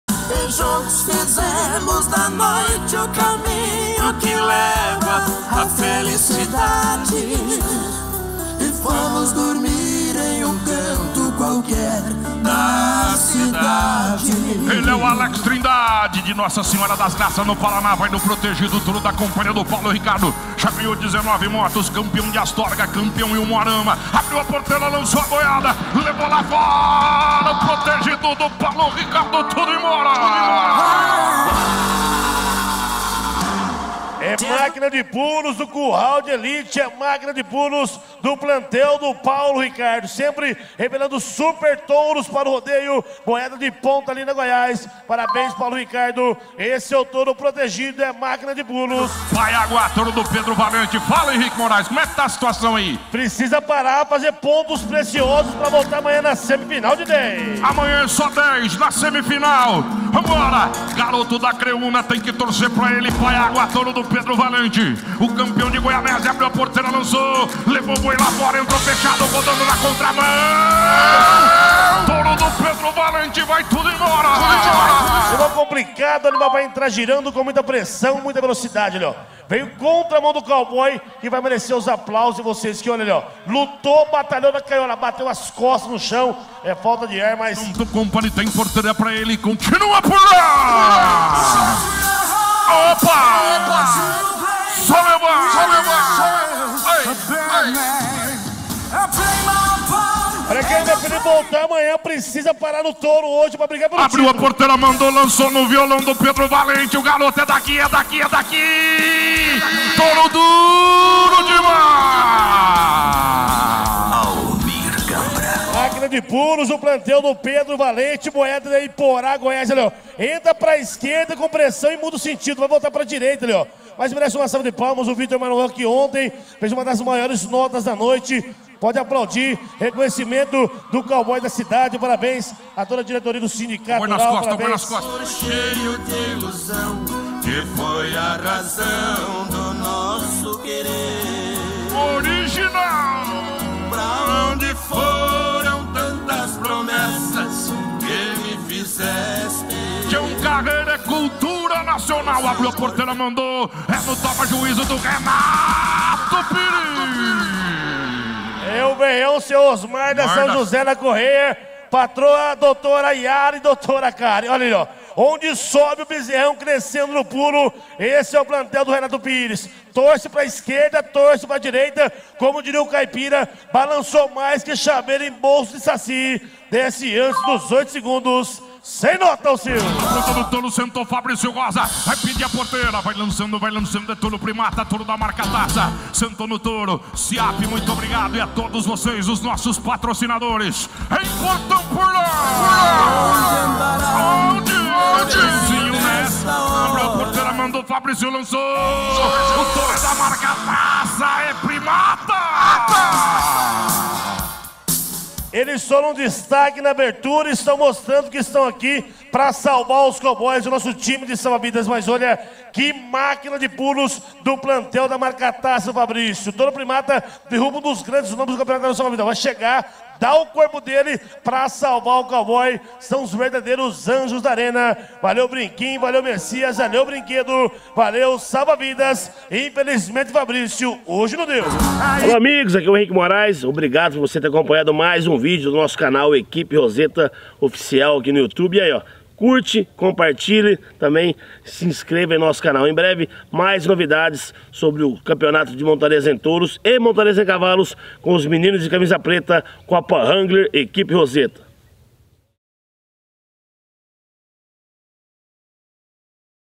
ACREÚNA-GO 2022 | SÁBADO / MELHORES TOUROS
16ª EXPOSIÇÃO AGROPECUÁRIA DE ACREÚNA-GO 2022
23ª ETAPA COPA WRANGLER ROZETA

SÁBADO / MELHORES TOUROS

00:08 • Protegido - Cia. Paulo Ricardo | Nota Individual: 44,50
00:50 • Payaguá - Cia. Pedro Valente | Nota: 44,75
01:28 • Violão - Cia. Pedro Valente | Nota: 44,75
02:16 • Toma Juízo - Cia. Renato Pires | Nota: 45,00
03:01 • Primata - Cia. Marca Taça / Fabricio | Nota: 45,00

Edição e Imagens: Guilherme Ferreira